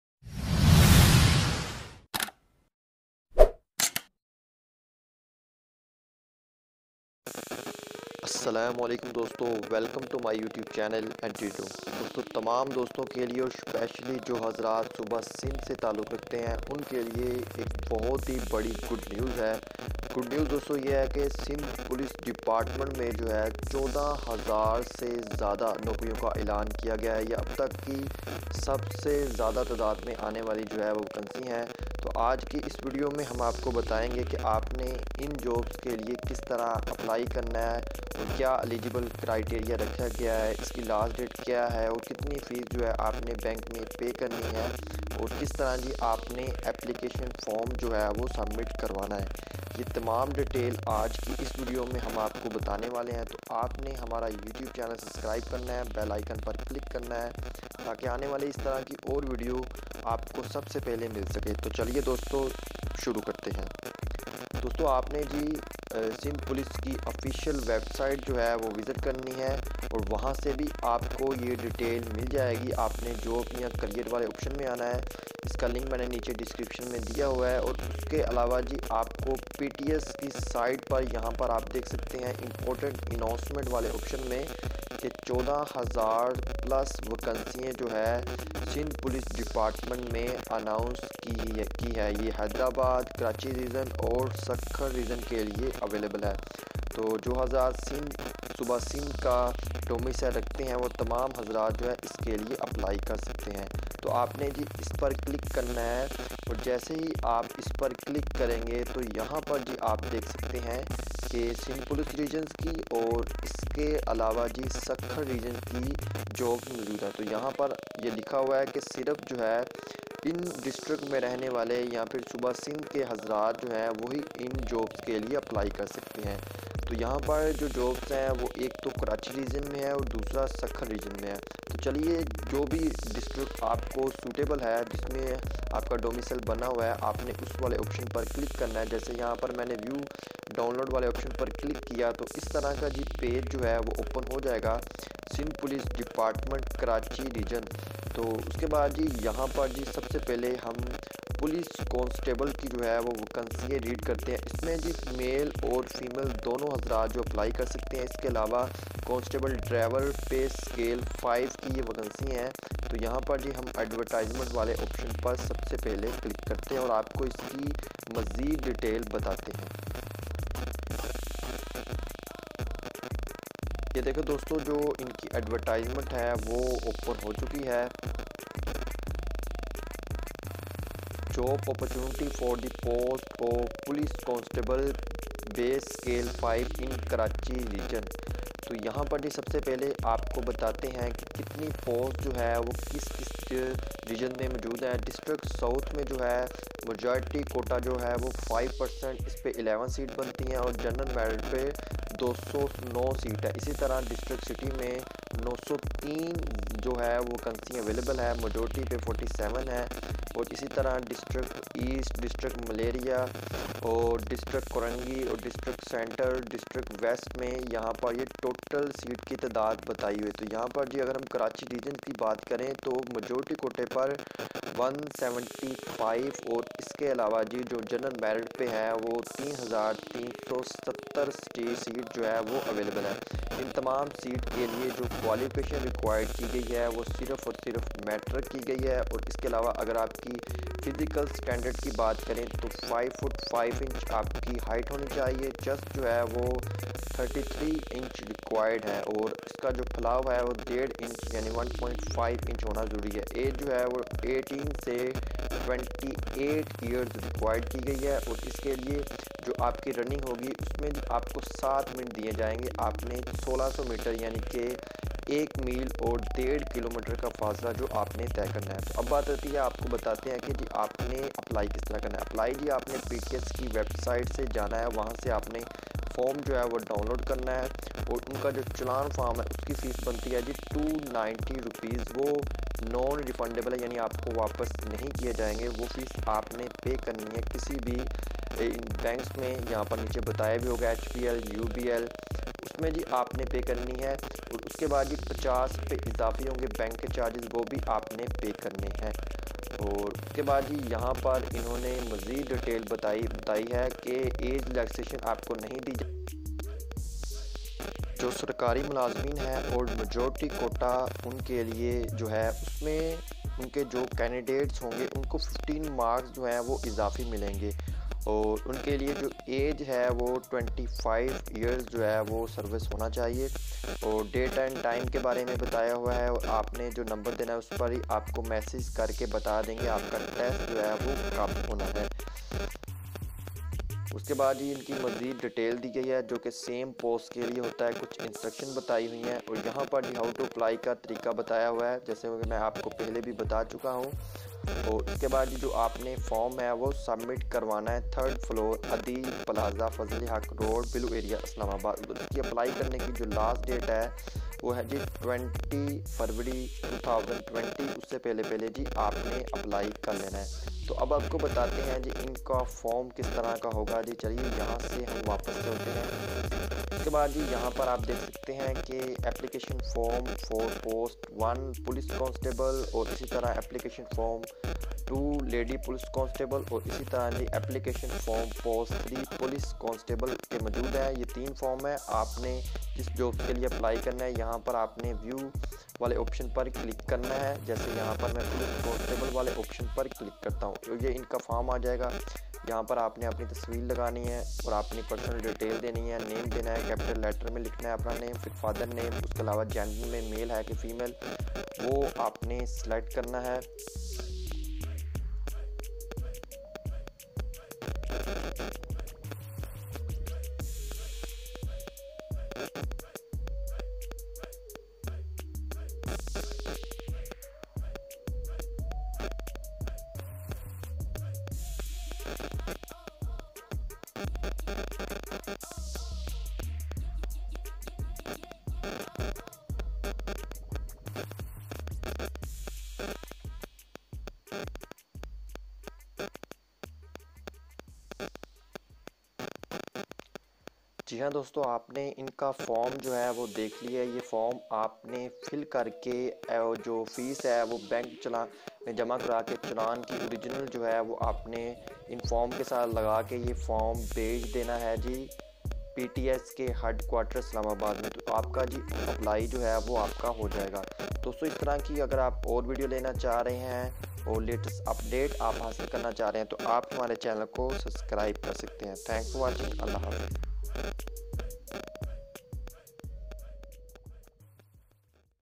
PTS Latest Jobs | Sindh Police Jobs 2020 | How to Sindh Police Form Complete Details
Please visit the link below to Online Download Application Form for Sindh Police Jobs 2020 February for Constable Drivers: